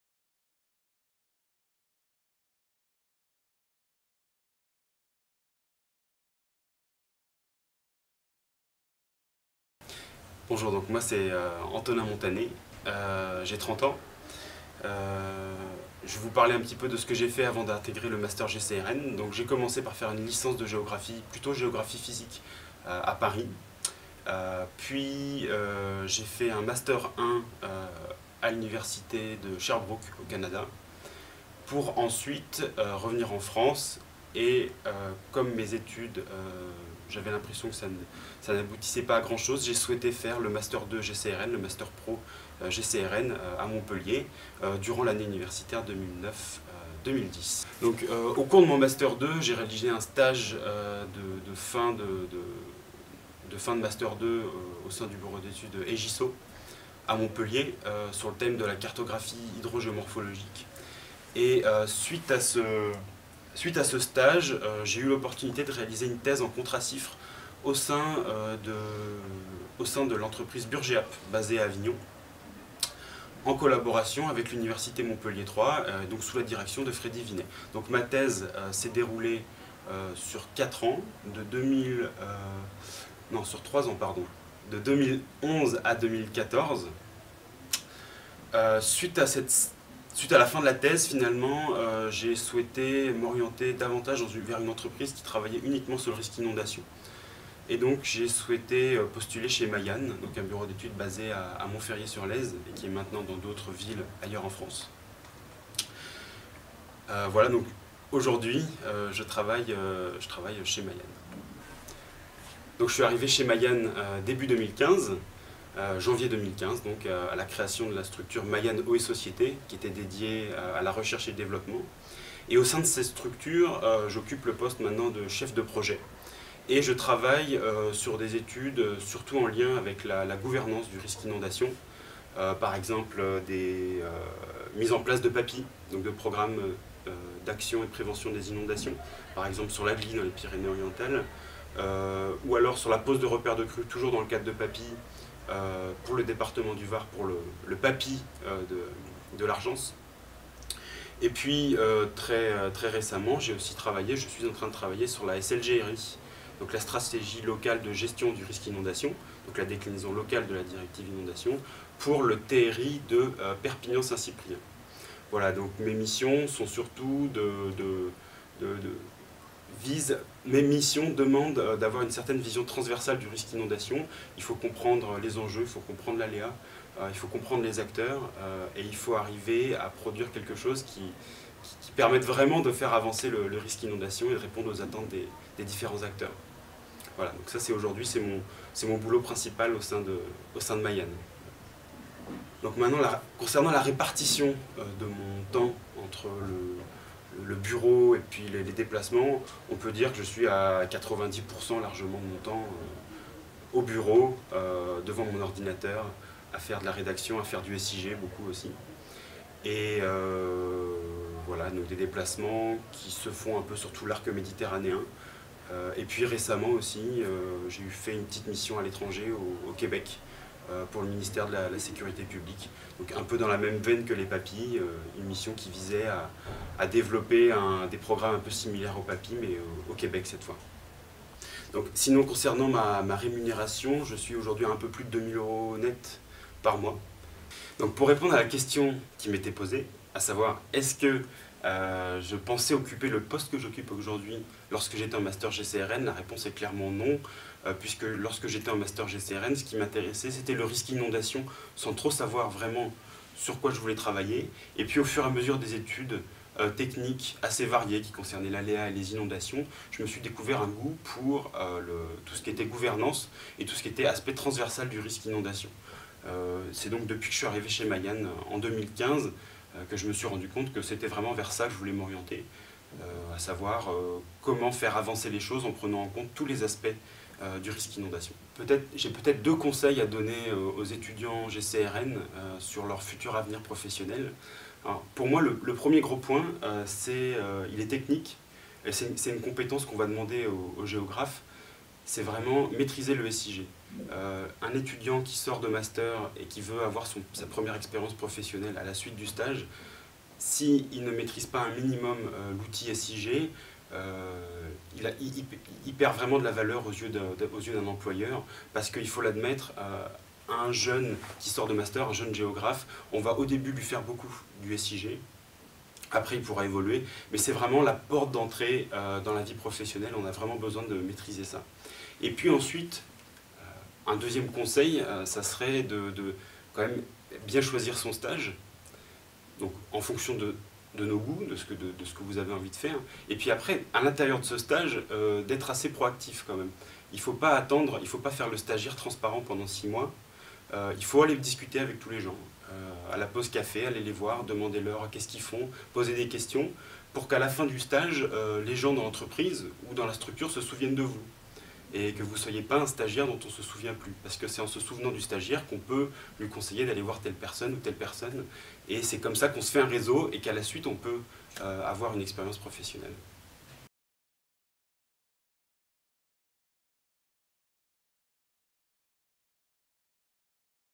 Interview d'Antonin Montané, ancien étudiant GCRN
Cette interview permet de montrer ce qu'il est possible d'envisager comme parcours professionnel à la suite de la formation en Master Gestion des Risques et des Catastrophes Naturelles.

Université Paul-Valéry - Montpellier 3 - 2017